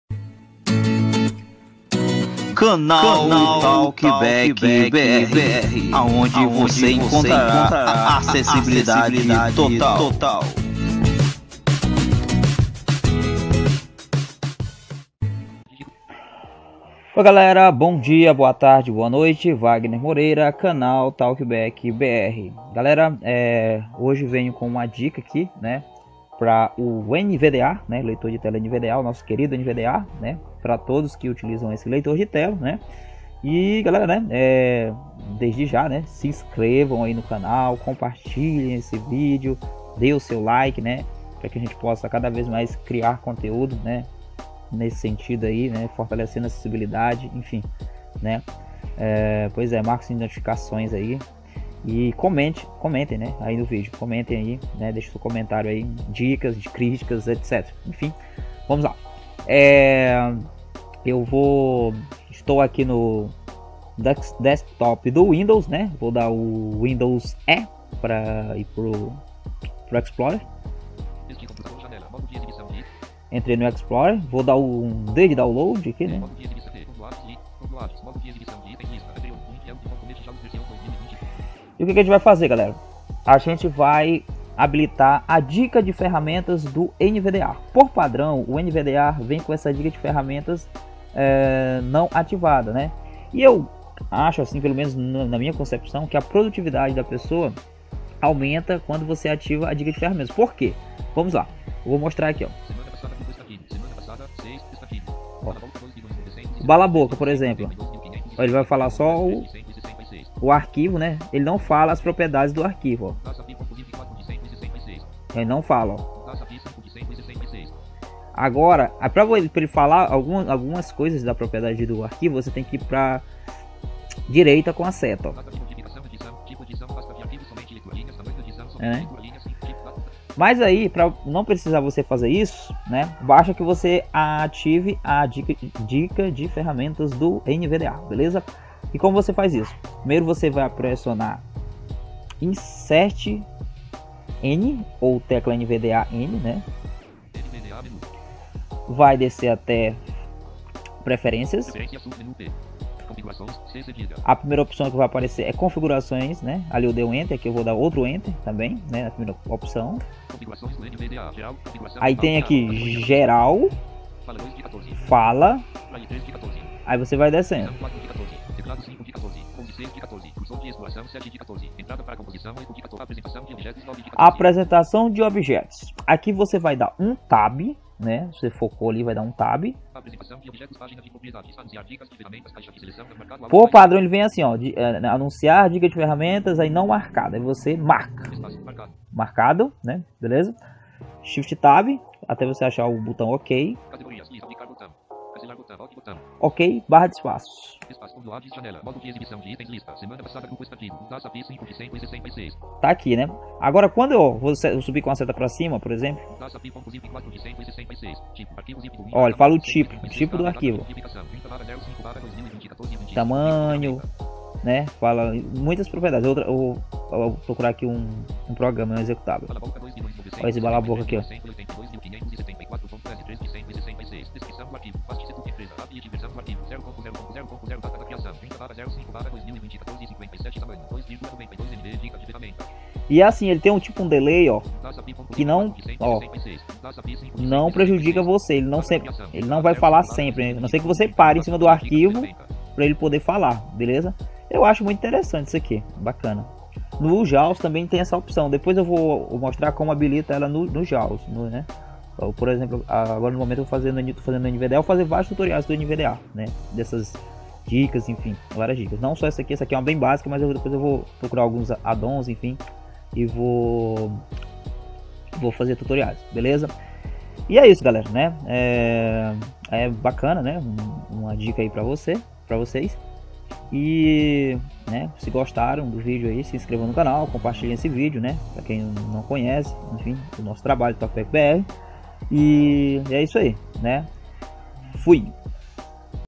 Uma ótima função do (NVDA), ative já! vai melhorar muito a sua produtividade com o leitor!
oi galera!
hoje venho com esse tutorial para você melhorar sua produtividade com o leitor de telas NVDA, assistam  e confiram...